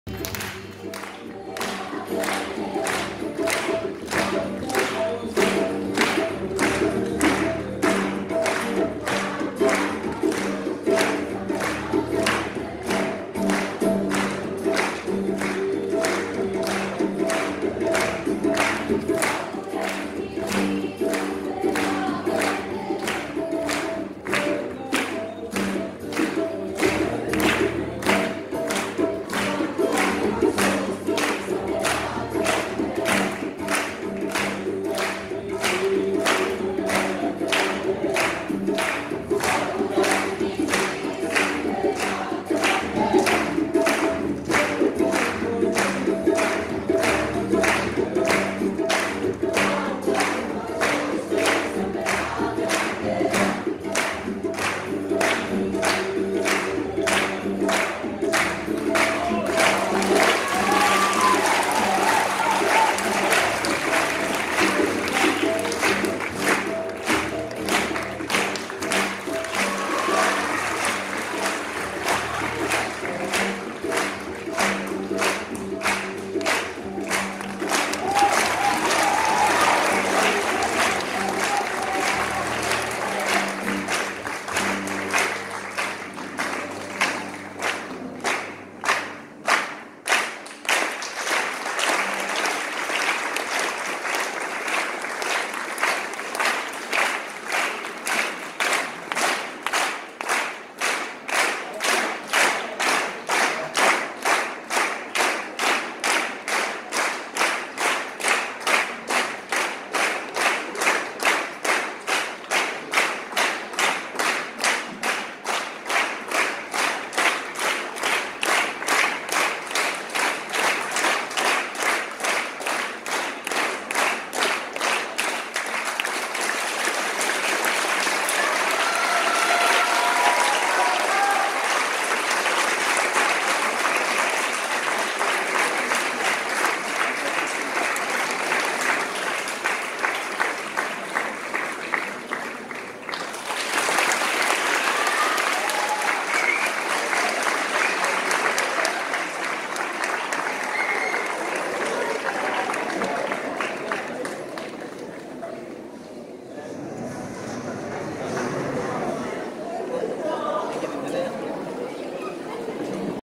Curtain Call - 'නාග මණ්ඩල' අධ්‍යක්ෂණය ප්‍රියංකර රත්නායක සහ සංජීව ගුණරත්න 2023 ජනවාරි 10 සවස 4ට
කෝඩුකාර නාට්‍ය උළෙල 2023
කැළණිය විශ්වවිද්‍යාලයීය නාට්‍ය හා රංගකලා වසර නාට්‍ය නිෂ්පාදනය
‘නාගමන්ඩල’
අධ්‍යක්ෂණය ප්‍රියංකර රත්නායක සහ සංජීව ගුණරත්න
2023 ජනවාරි 10 අගහරුවාදා සවස 4ට
කැලණිය විශ්වවිද්‍යාලයීය ධර්මාලෝක ශාලාවේදී

A Sinhala translation of Indian playwright Girish Karnad's 1988 play 'Naga-Mandala' (Play with Cobra) based on a folk tale related to him by A. K. Ramanujam, presented by the Drama & Theatre degree students, directed by Priyankara Rathnayake & Sanjeewa Gunarathne, and performed for the Amateur Theatre Festival 2023 at 4 pm on Tuesday, 10th January 2023 @ Sri Dharmaloka Hall, University of Kelaniya.

Drama Sri Lanka

Get Drama Alerts by SMS!